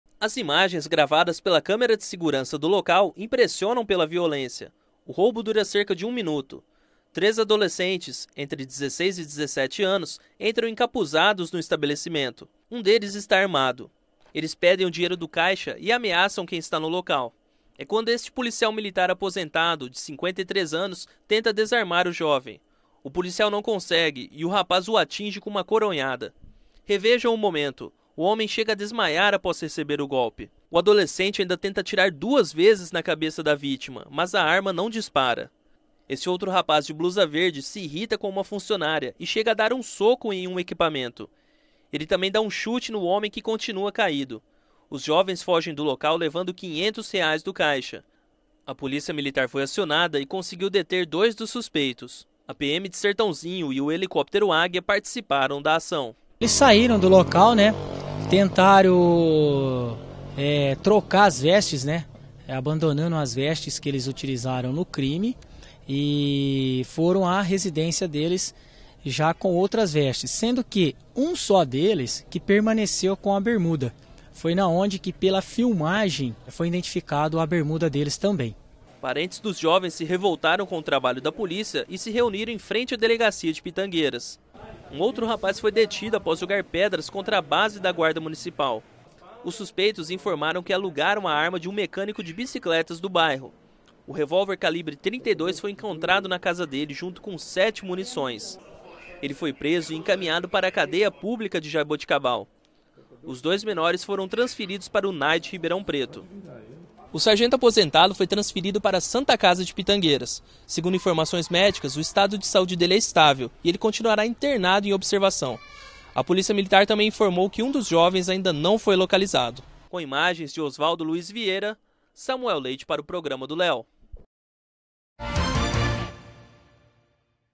Arma falha e PM aposentado escapa da morte durante assalto
Um policial militar aposentado foi espancado por três jovens armados durante um assalto a um supermercado, na tarde de sábado, em Pitangueiras - SP. As imagens registradas pela câmera de segurança do estabelecimento mostram que o PM só não morreu porque a arma de um dos assaltantes, que tentou por duas vezes atirar na cabeça
 dele, falhou. Quatro dos cinco envolvidos no crime - dois deles menores de idade - foram detidos.